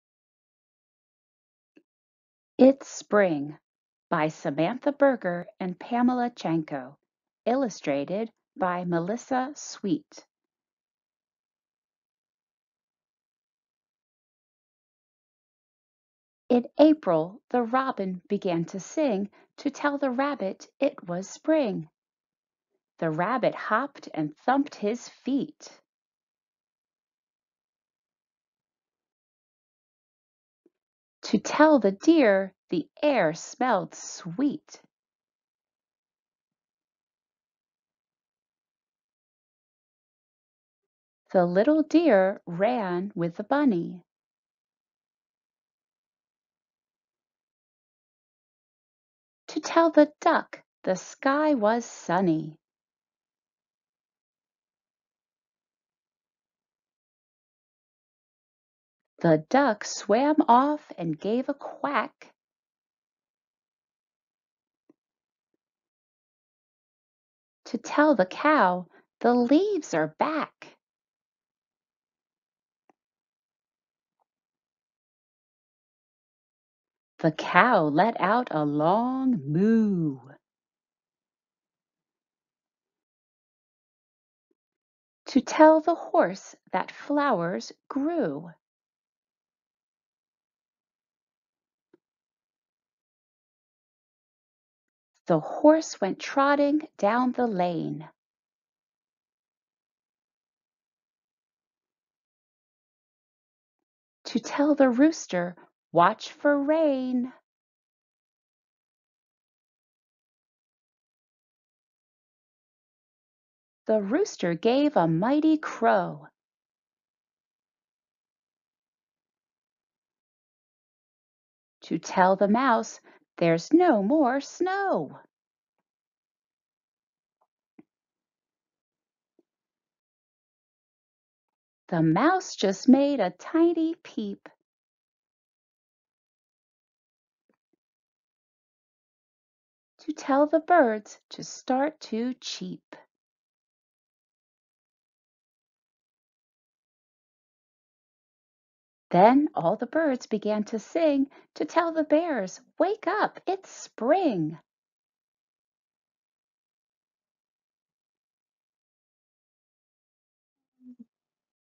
It's Spring!
A "Hello Reader" level two book. If you are teaching about the signs of spring and would like to incorporate sequencing, this book would work great. Meet all of the animals that have to tell each other that spring has come. Can you remember the order that the animals appeared?
Written by Samantha Berger and Pamela Chanko, illustrated by Melissa Sweet.
This book is intended for non-profit educational use only. Available for purchase